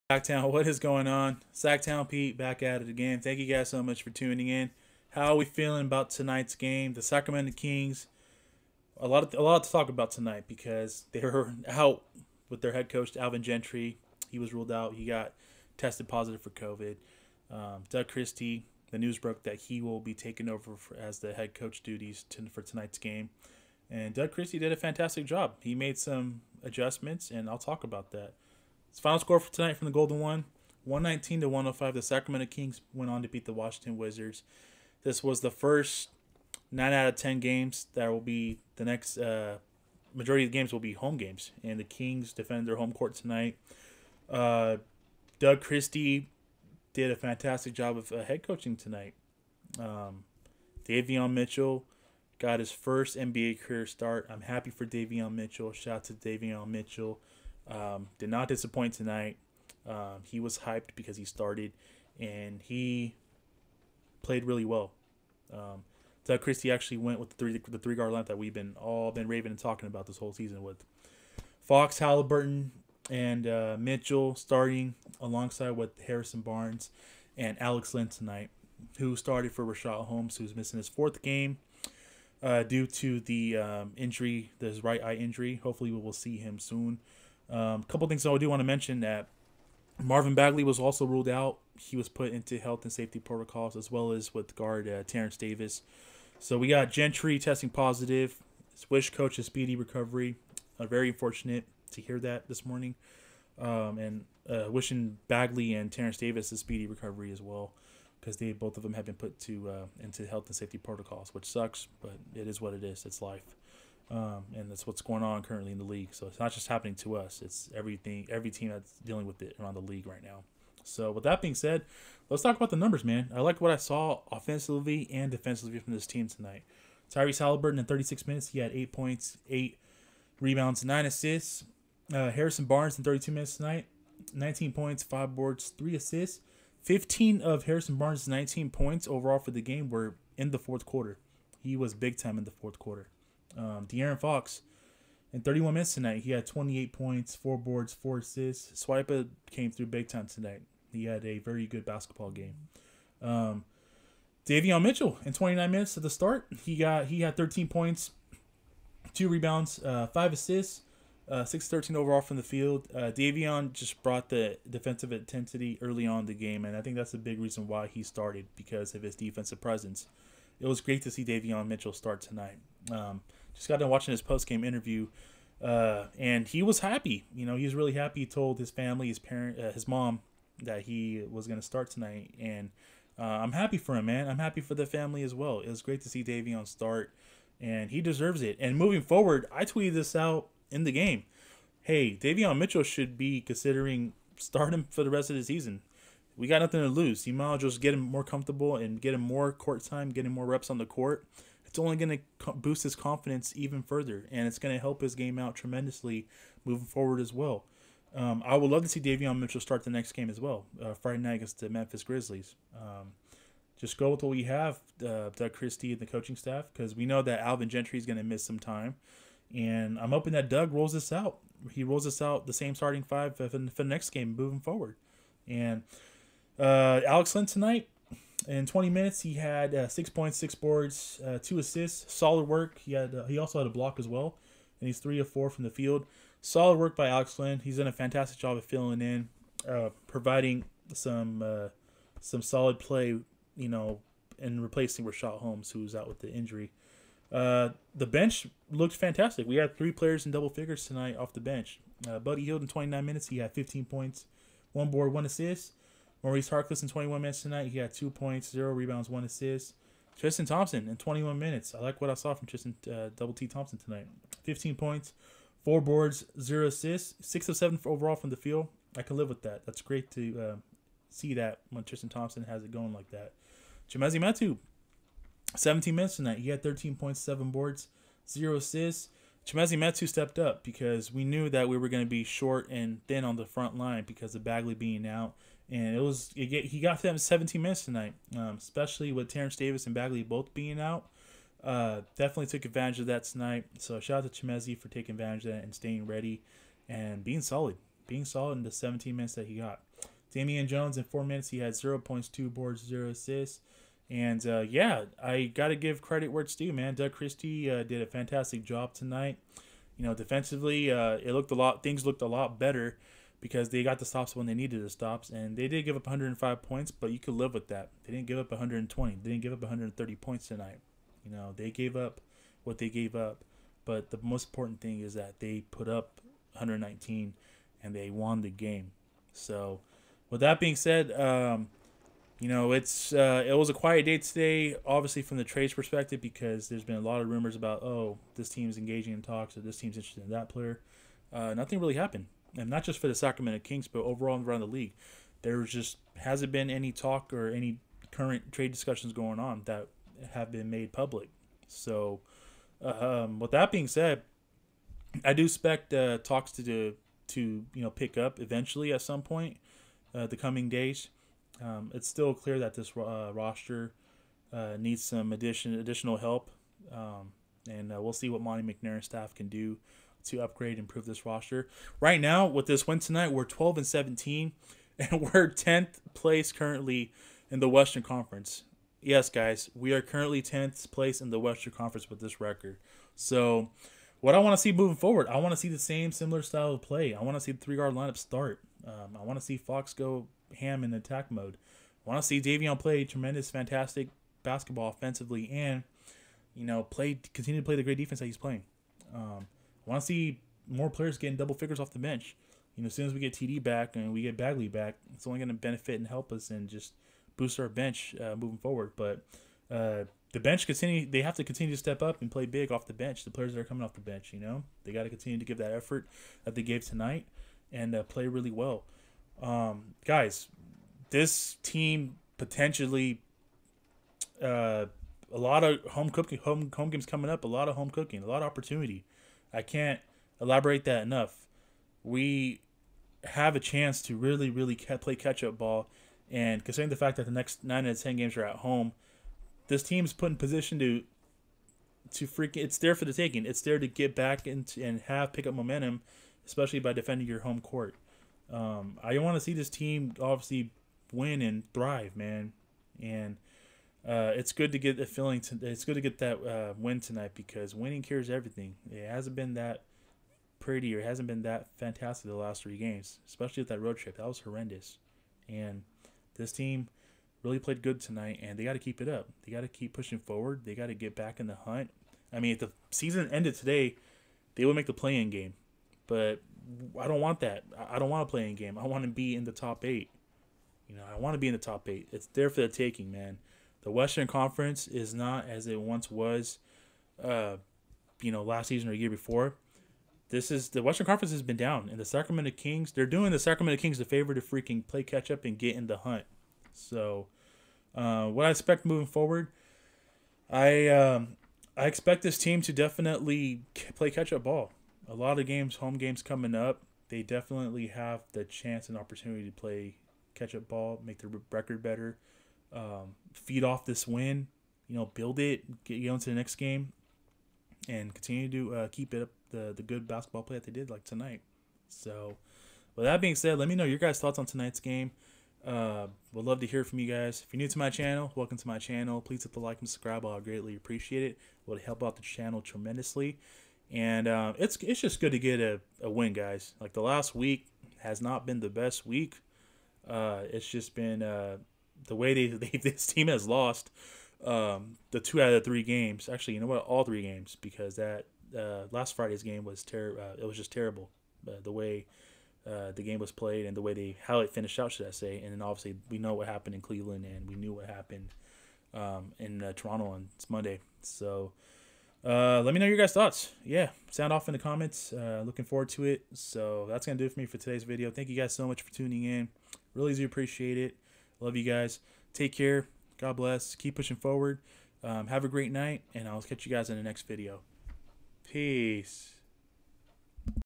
START Davion Mitchell rest of the season!! Kings win
Davion Needs to start moving forward!!
Great job by Doug Christie